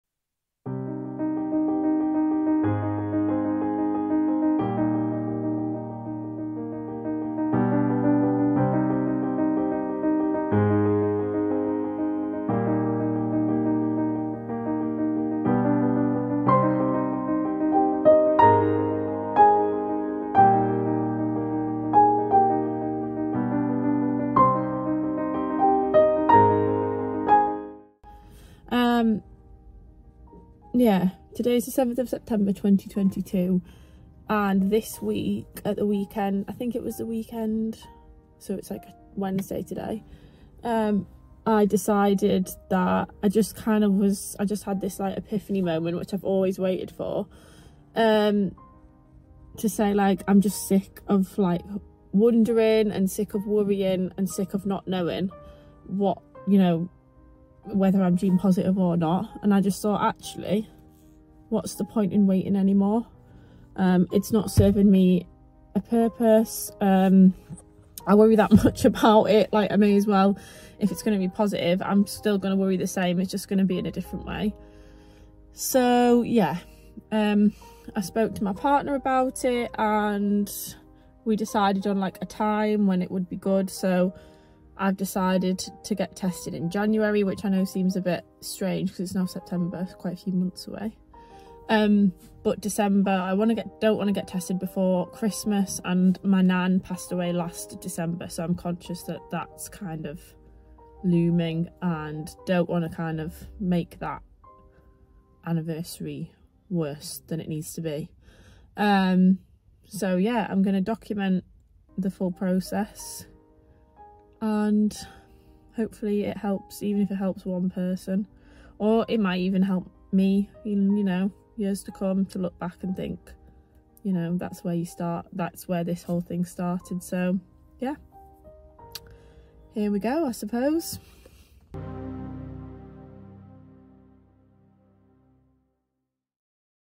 Charlotte's Testing Story: Deciding to Test
In September 2022, Charlotte decided it was time for her to go through genetic testing. She worked with her genetic counselor to schedule.